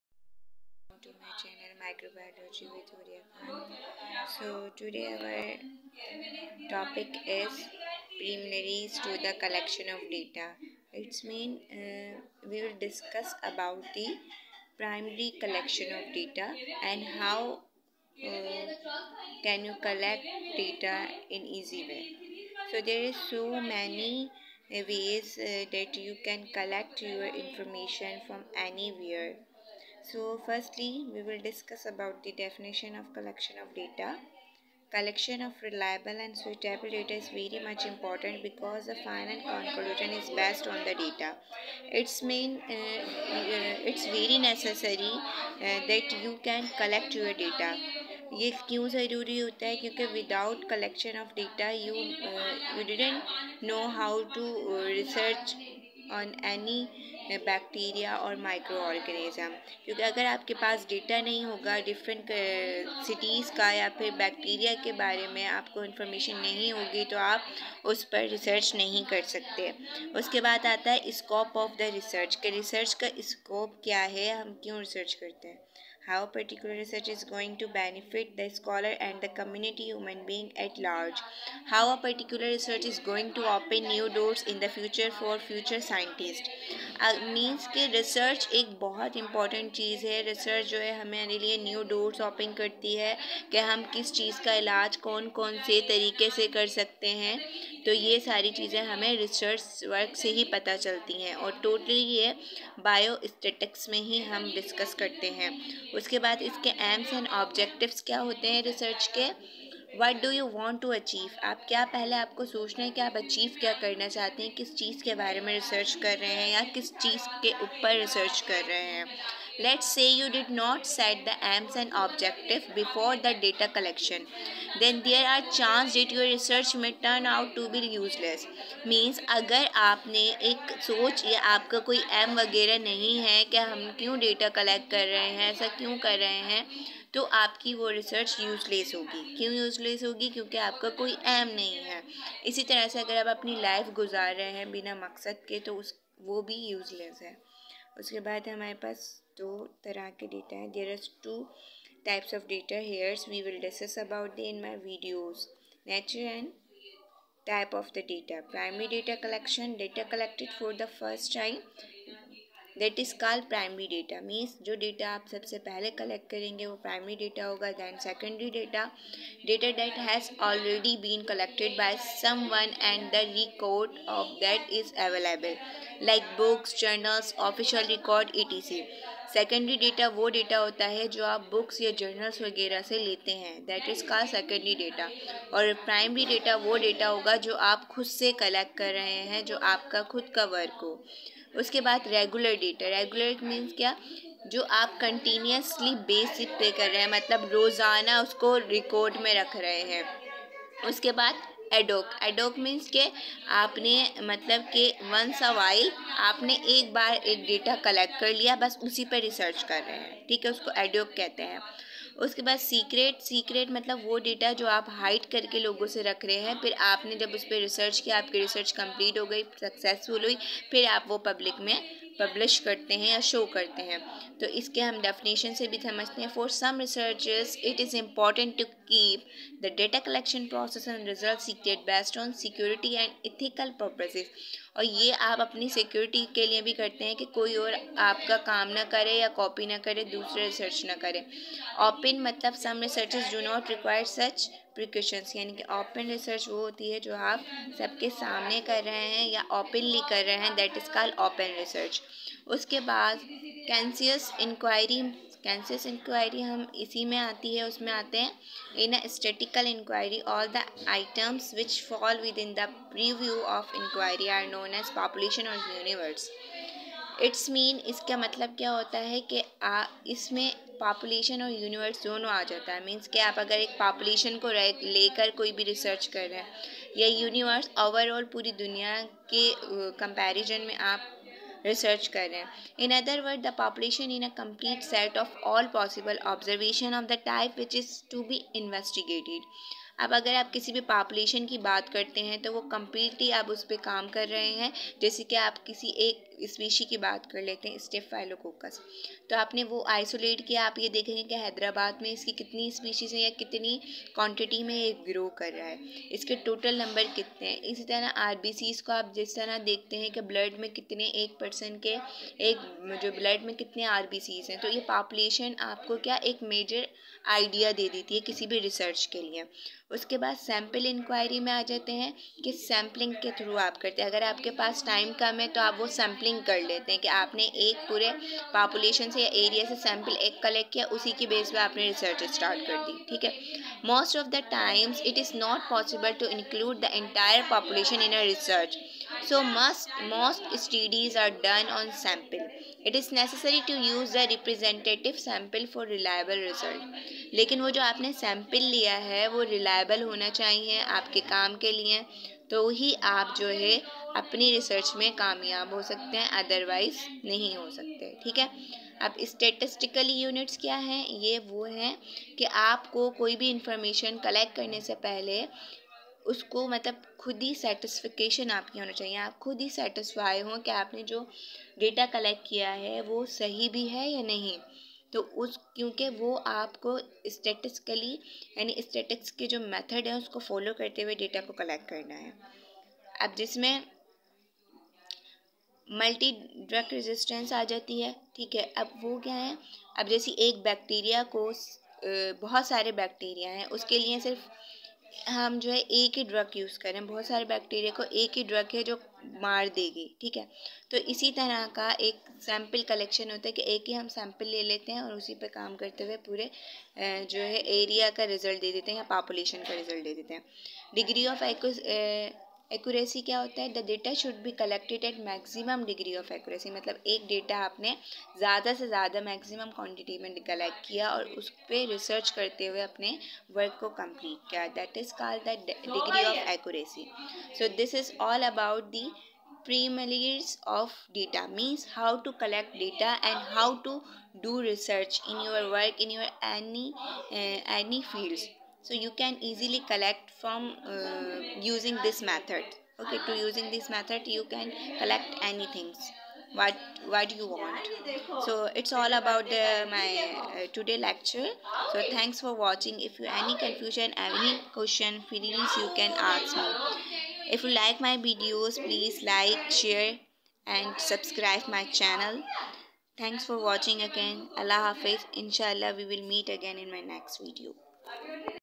DAta collection lecture 2
in this video u will watch how to collect data from anywhere. and how u can research on your project .
Thanks
Huriya khan